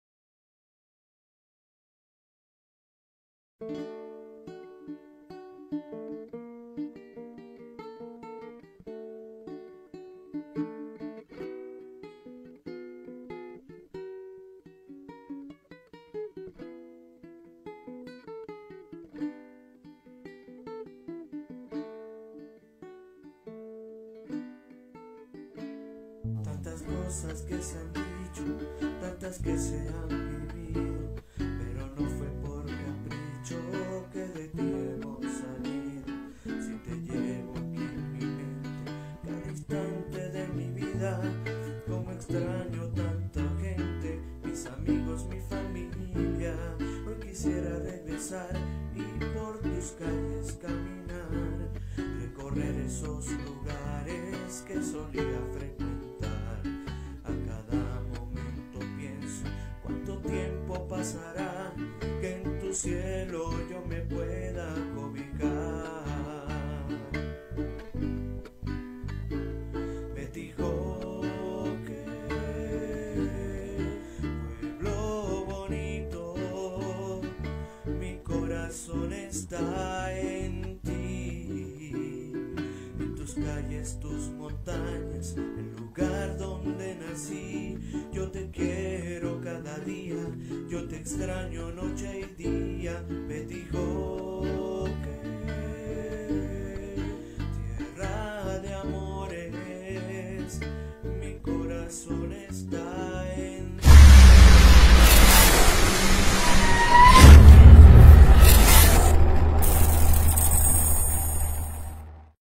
En la nieve con mis personajes Retratos y Dibujos en acuarela
Desde Venezuela para el mundo, la Capsula presente, con sus personas y su pueblo Betijoque